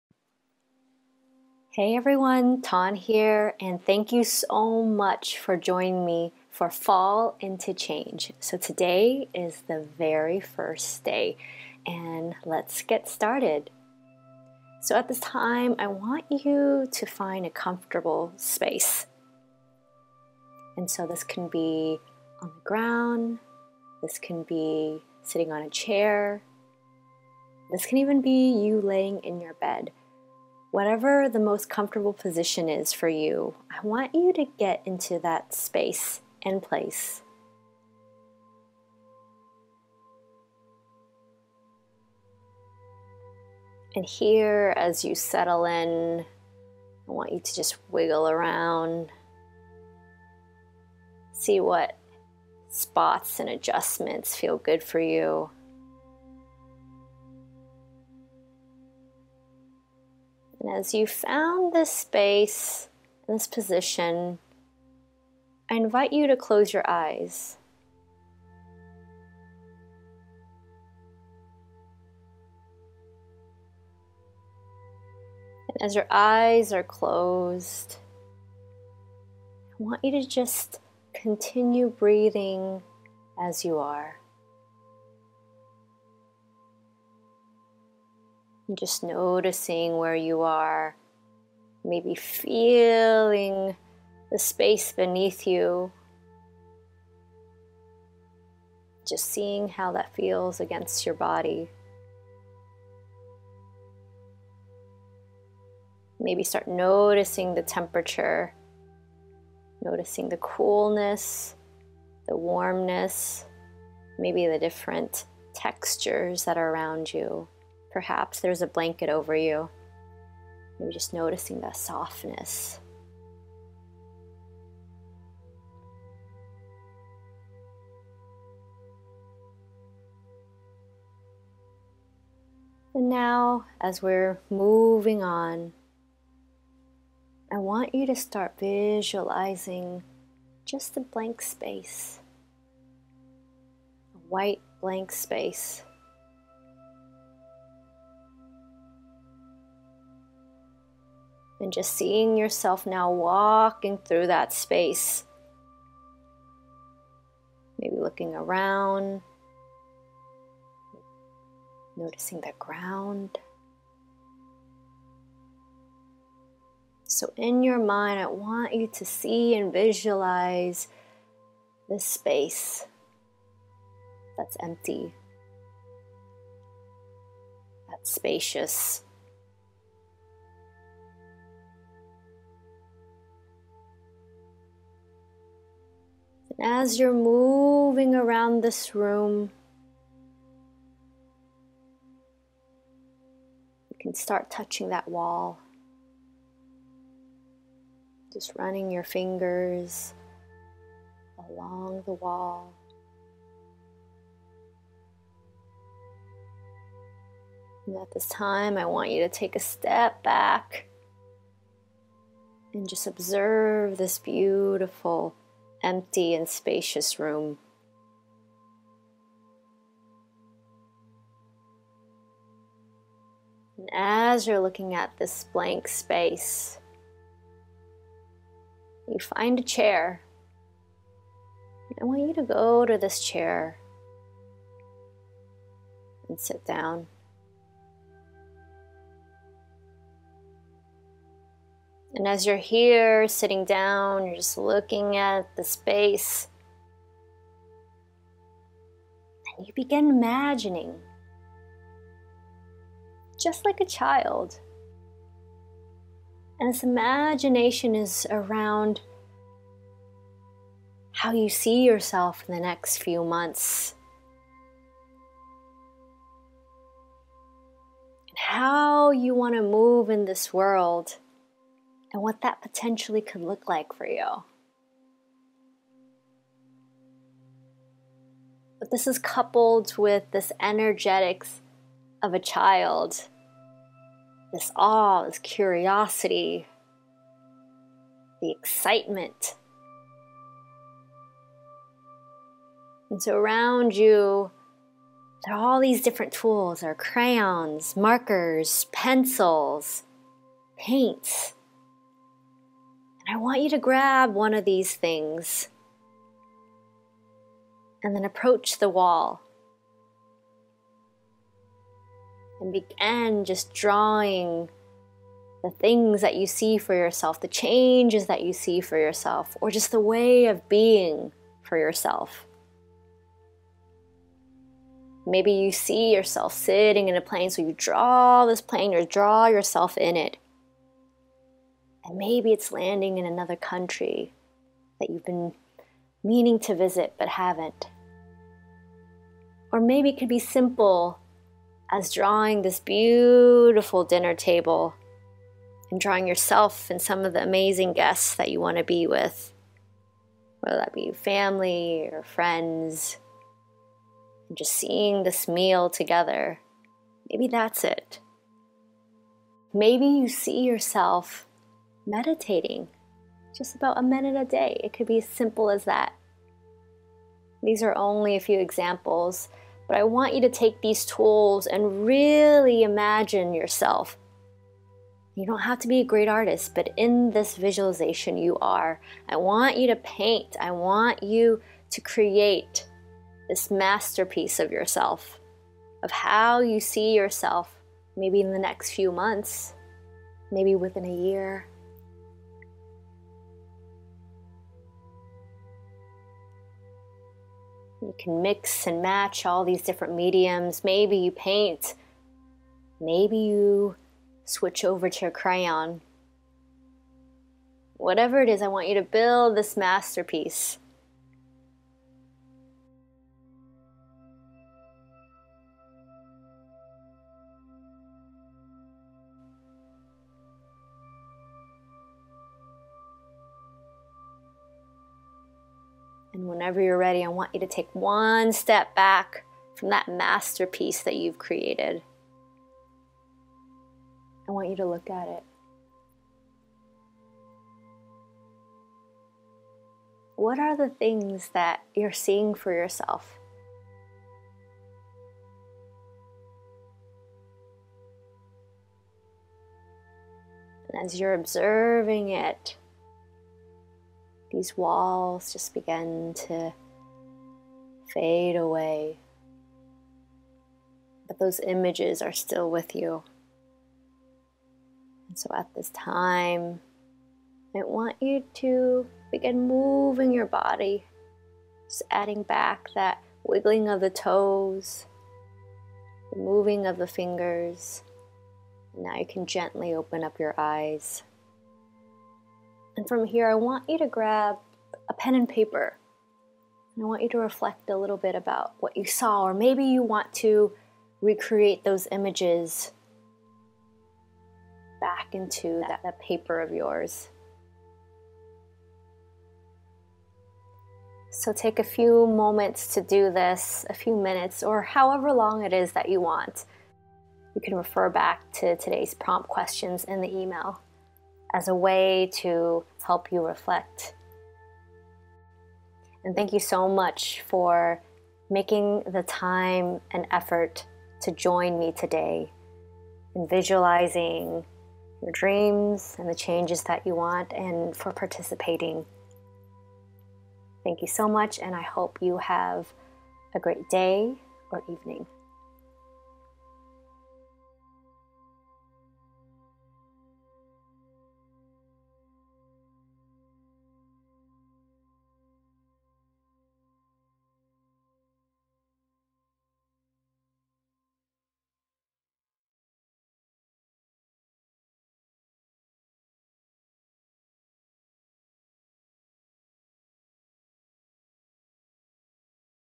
☁️ Visualize your Dream Guided Meditation
This is a short meditation that's part of the 7-Day 'Fall into Change' Challenge on Tawn-Le.com.